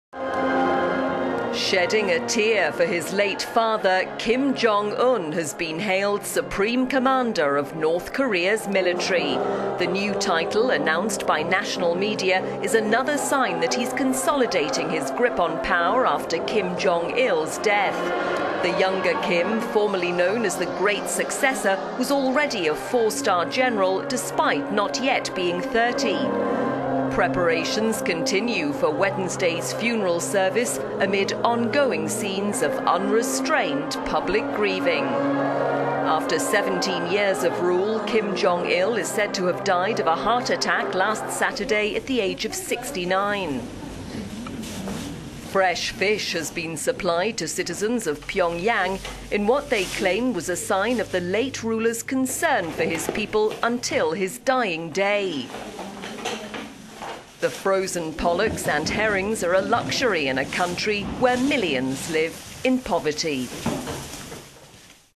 Kim Jong-un: North Korea's 'Supreme Commander'
Kim Jong-un has been hailed 'Supreme Commander' of North Korea's military.

The new title, announced by national media, is another sign that he is consolidating his grip on power after Kim Jong-il's death.

The younger Kim, formerly known as the 'Great Successor', was already a four-star general despite not yet being 30.

In newly-released pictures, he is seen shedding a tear by the body of his father which is lying in state until Wednesday's funeral service.